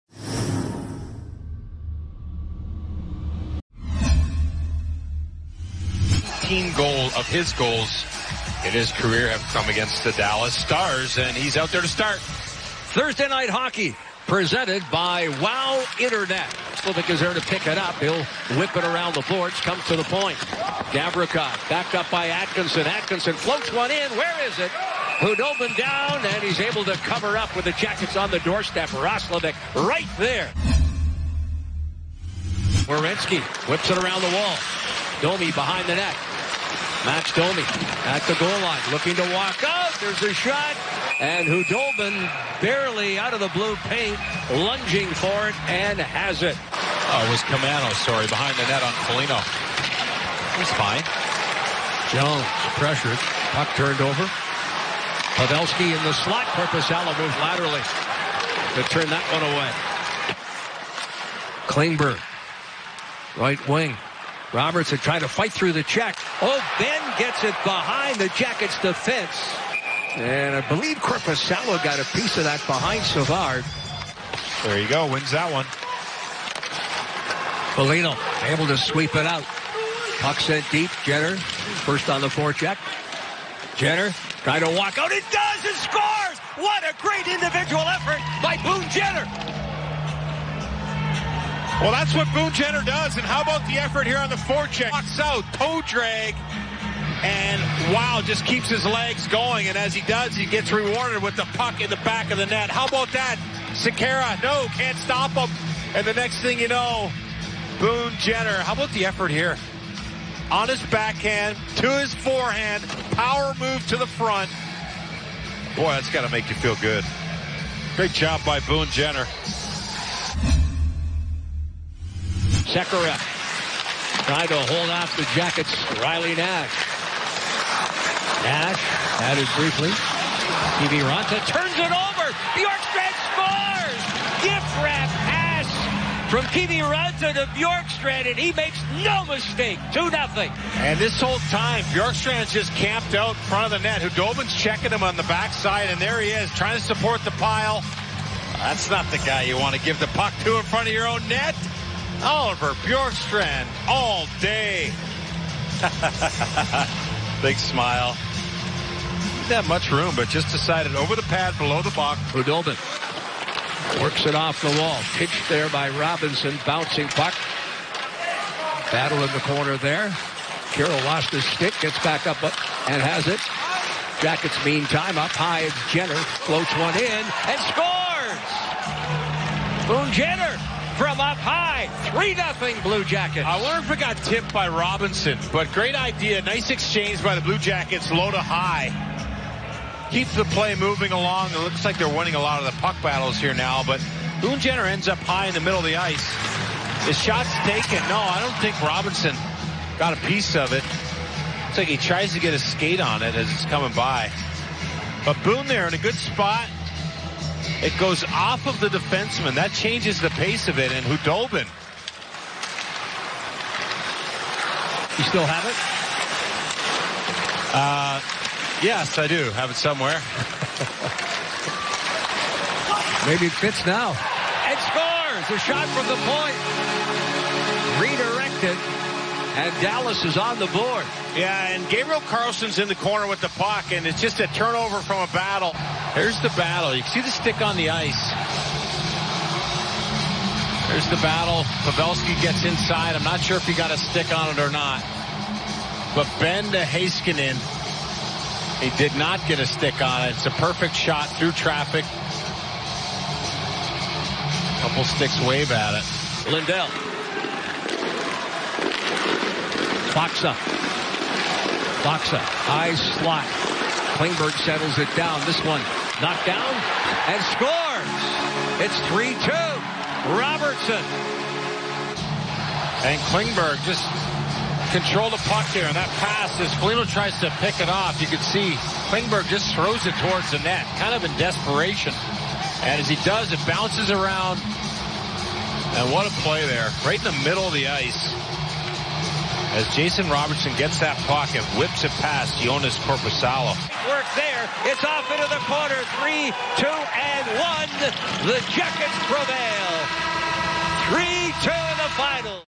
Game Highlights: CBJ 3, DAL 2 (3/4/21)
The Columbus Blue Jackets win their second straight with a 3-2 win over the Dallas Stars on Thursday, March 4, 2021 in Dallas, Texas at American Airlines Arena.